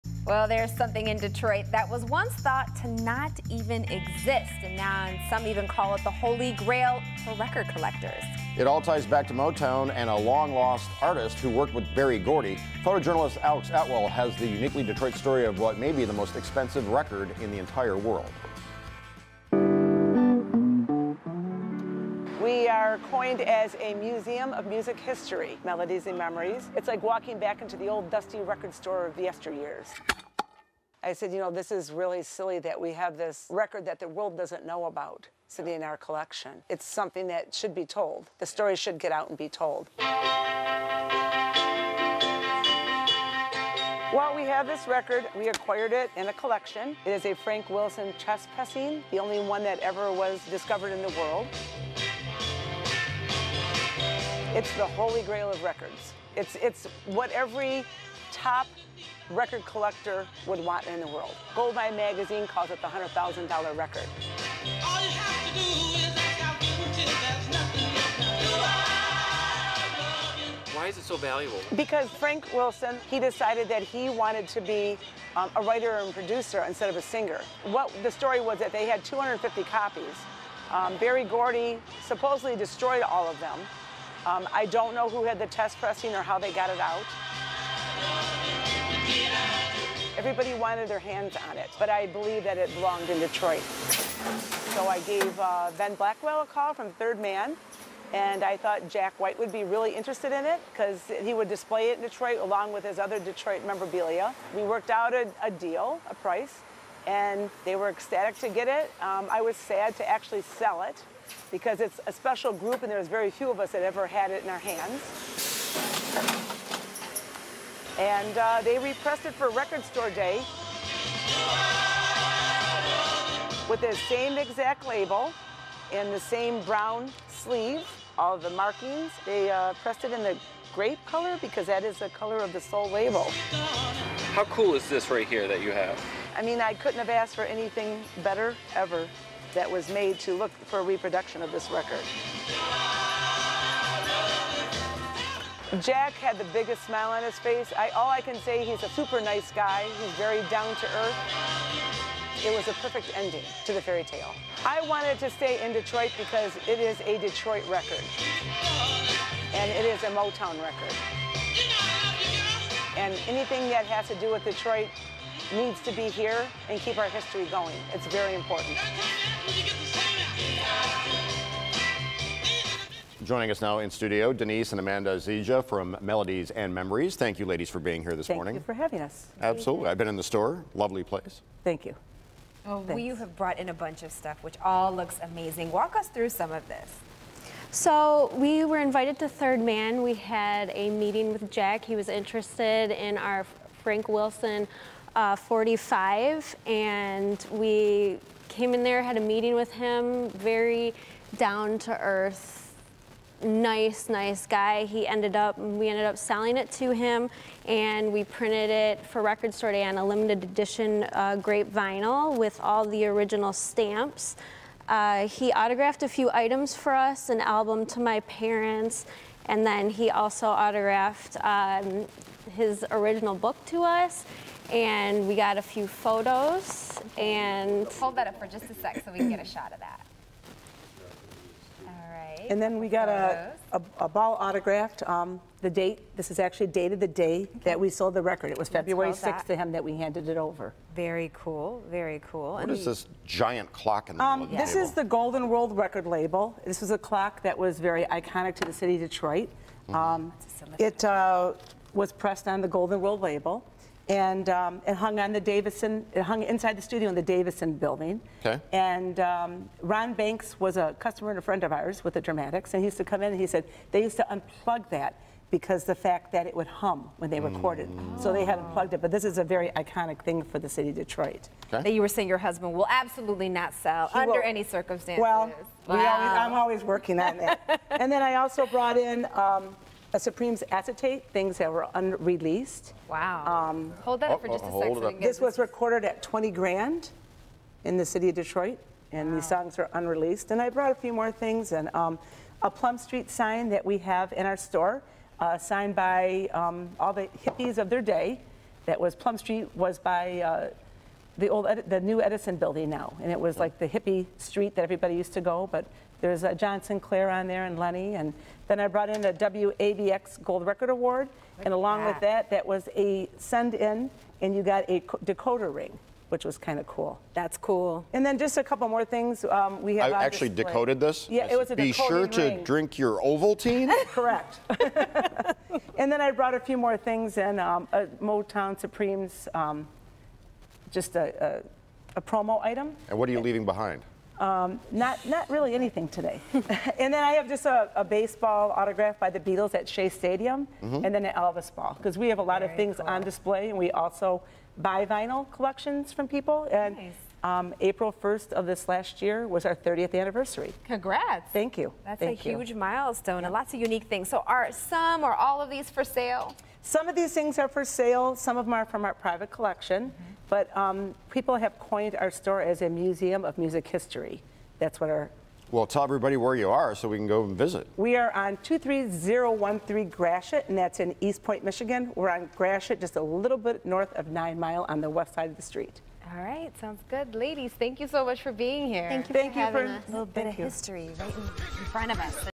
Live in the D: Uniquely Detroit - Unique finds at melodies and memories
It's a piece of music history that's one of a kind in the world because everyone thought it was destroyed!